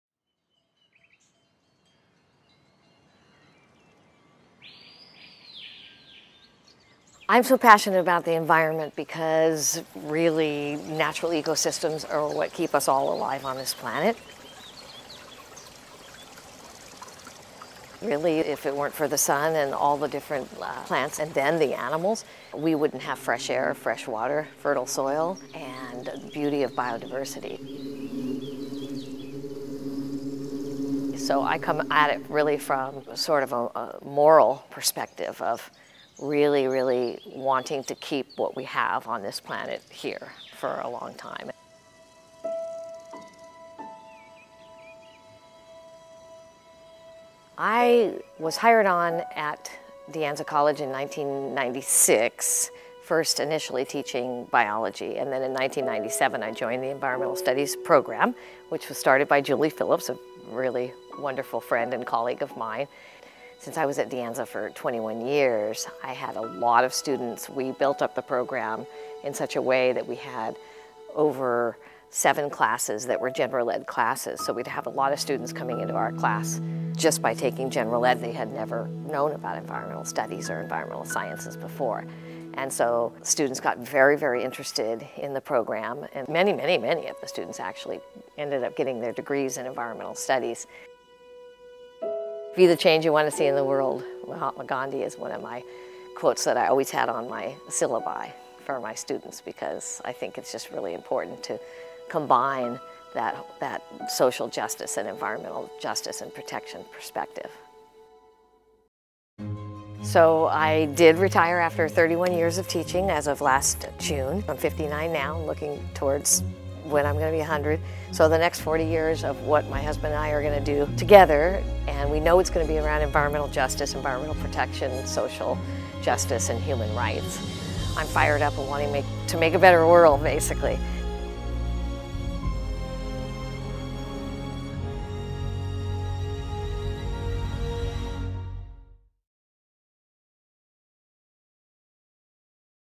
CREST Awards 2018: Kristin Jensen Sullivan (Sustainability Champion of the Year)
Kristin Jensen Sullivan was an environmental studies instructor at De Anza College for 21 years before retiring in mid-2017. During her tenure at De Anza College, Kristin motivated and inspired hundreds of students and community members to get involved in climate protection and environmental justice issues. The City of Cupertino’s Sustainability staff had the opportunity to work with many of Kristin’s environmental studies students to certify 15 De Anza College departments as certified green businesses and conduct visual trash audits to protect creeks from litter. Kristin’s leadership also led to meaningful reductions in water use, energy use, and waste at the college.